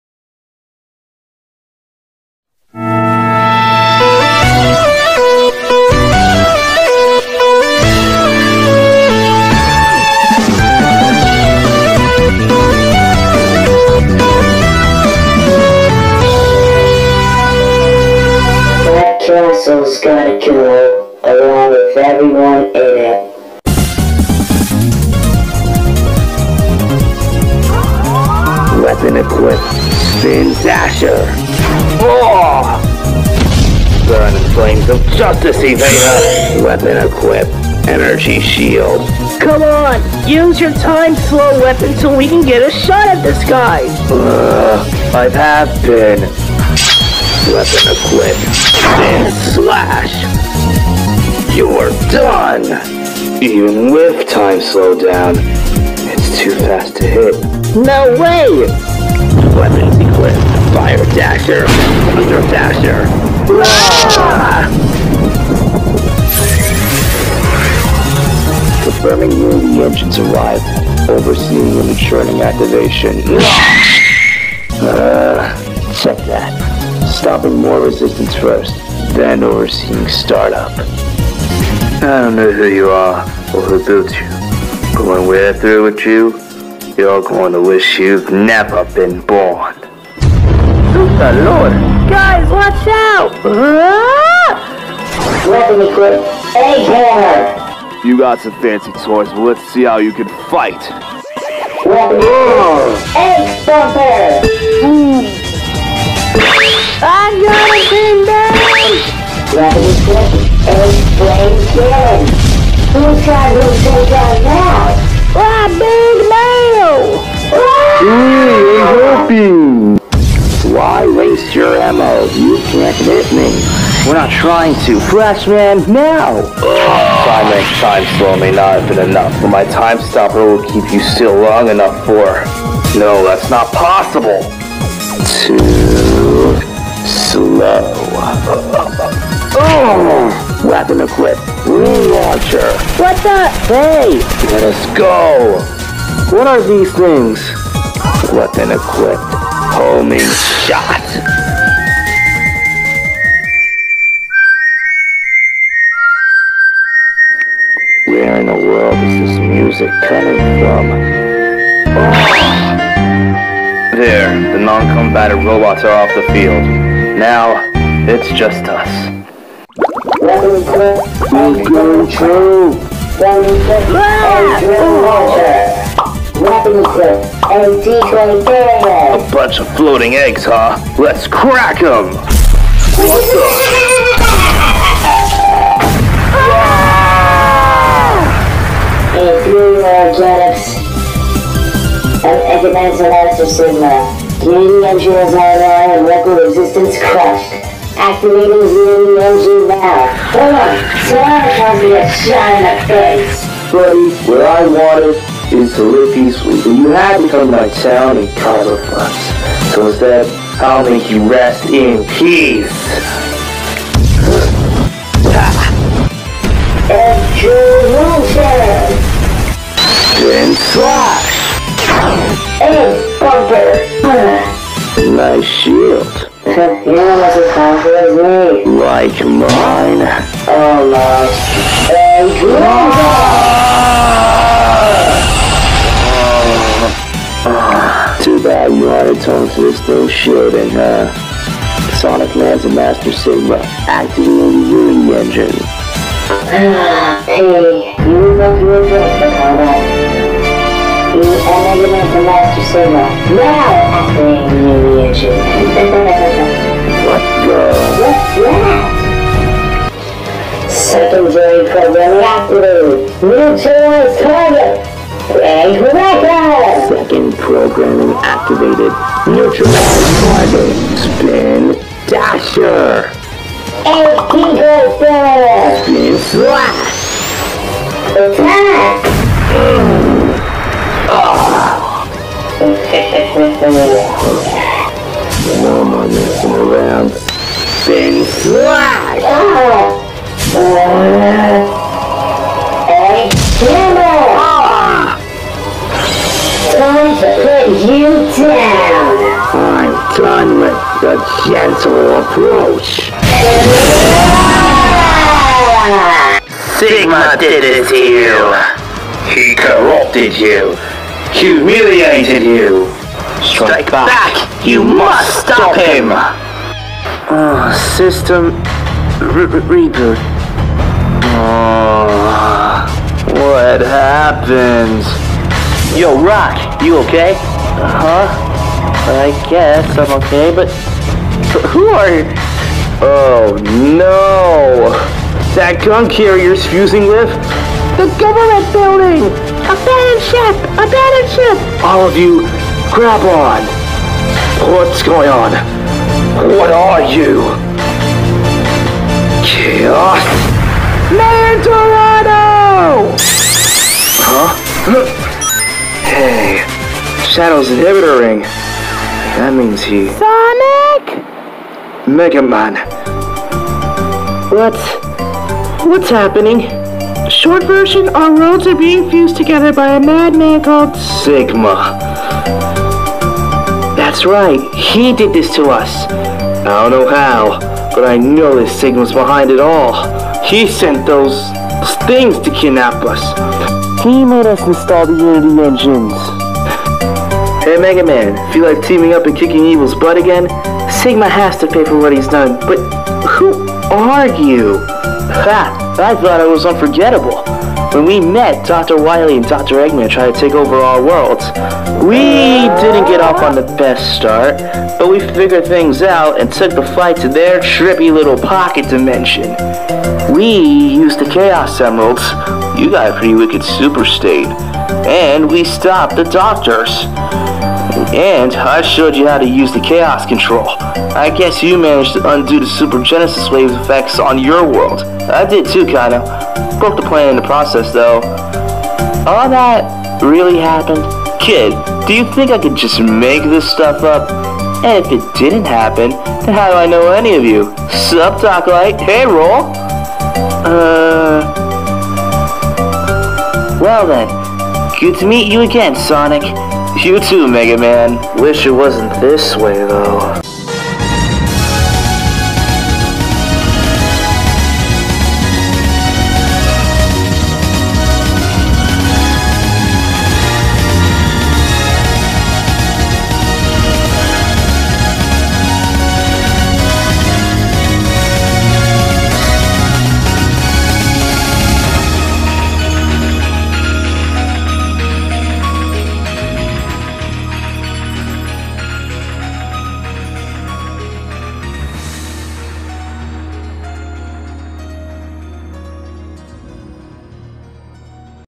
Sonic man and m'egga man vs everyone
This had to be one of the longest hardest dubs we ever did and we put a lot of effort into choosing the background music and the sound effects so hit that like button subscribe follow us on Instagram to watch videos that were blocked thanks to YouTube and enjoy the video